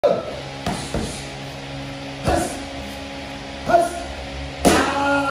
Kids kickboxing 🔥🥊
Padwork with one of our advanced kids 🔥🥊